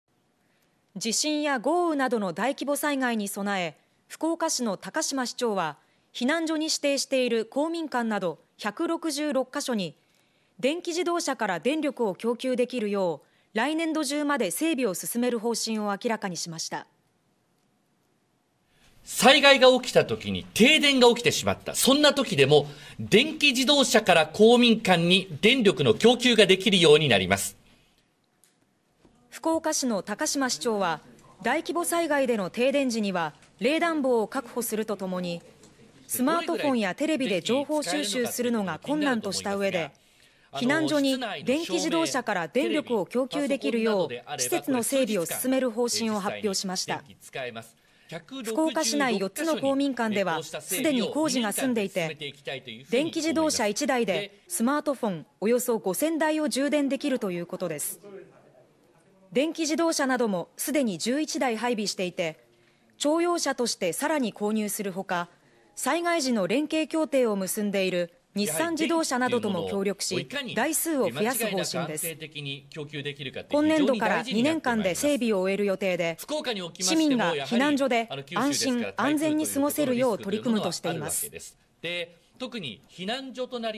避難所の非常用電源を電気自動車で確保…福岡市推進
地震や豪雨などの大規模災害に備え、福岡市の高島市長は２４日、避難所に指定している公民館など１６６カ所に、電気自動車から電力を供給できるよう、来年度中まで整備を進める方針を明らかにしました。

「災害が起きた時に停電が起きてしまった。そんな時でも、電気自動車から公民館に電力の供給ができるようになります」。

福岡市の高島市長は２４日午前の定例会見で、大規模災害での停電時には、冷暖房を確保するとともに、スマートフォンやテレビで情報収集するのが困難としたうえで、避難所に電気自動車から電力を供給できるよう施設の整備を進める方針を発表しました。

福岡市内４つの公民館ではすでに工事が済んでていて、電気自動車１台でスマートフォンおよそ５０００台を充電できるということです。電気自動車などもすでに１１台配備していて、庁用車としてさらに購入するほか、災害時の連携協定を結んでいる日産自動車などとも協力し、台数を増やす方針です。

今年度から２年間で整備を終える予定で、市民が避難所で安心・安全に過ごせるよう取り組むとしています。